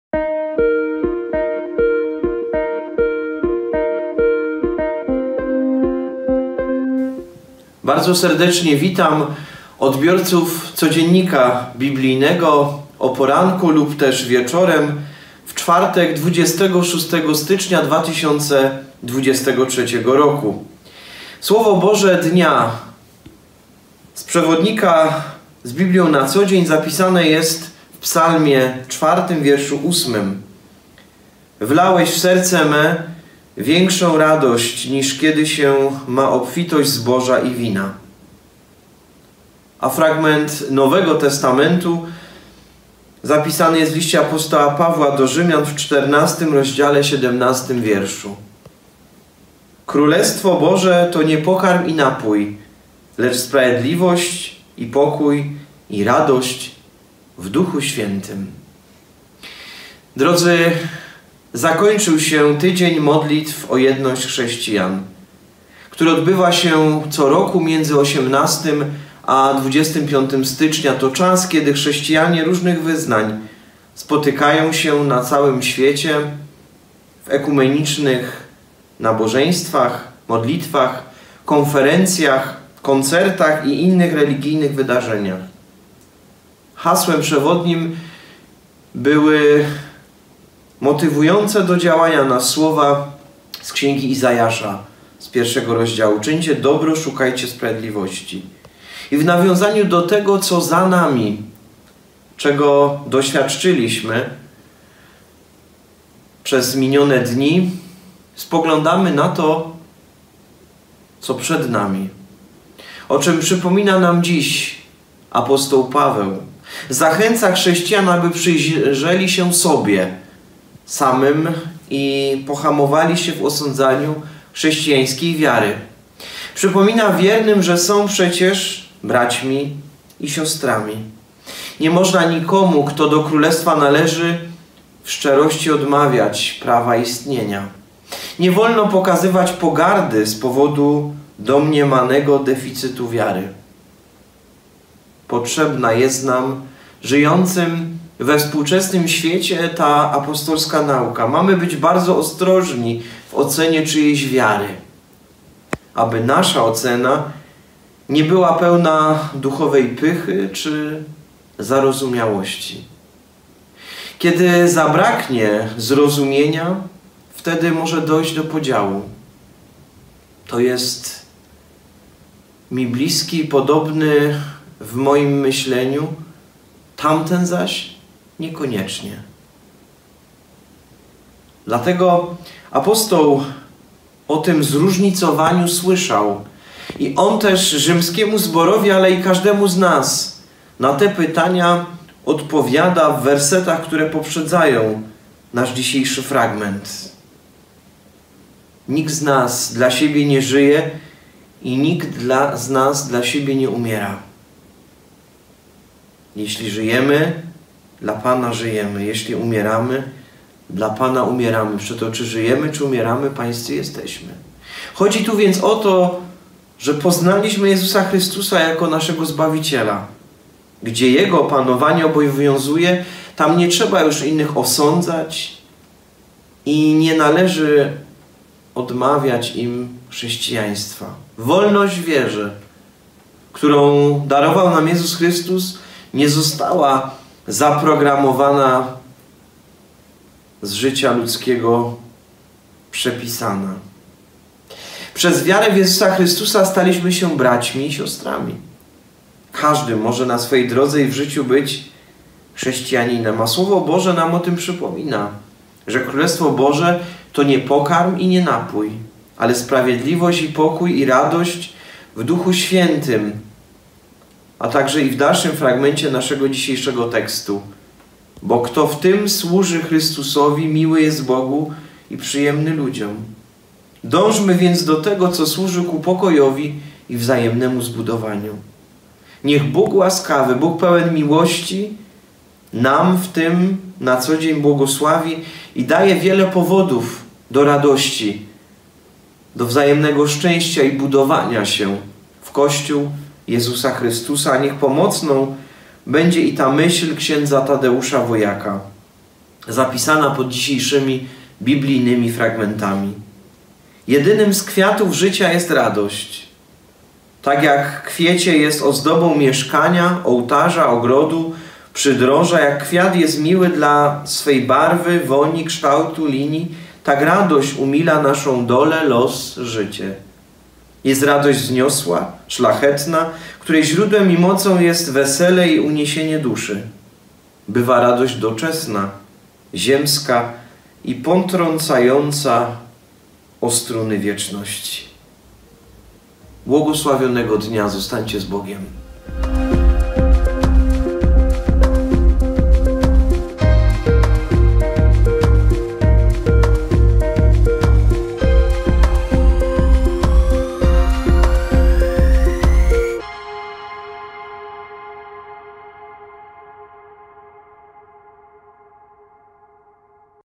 Codziennik biblijny, Słowo na dzień 26 stycznia 2023 r.
ks. Michał Matuszek, Parafia Ewangelicko-Augsburska w Ustroniu
Rozważanie biblijne na Słowo dnia:

Wlałeś w serce me większą radość, niż kiedy się ma obfitość zboża i wina.

Królestwo Boże, to nie pokarm i napój, lecz sprawiedliwość i pokój, i radość w Duchu Świętym.
Codziennie o 7 rano spotkanie z Biblią.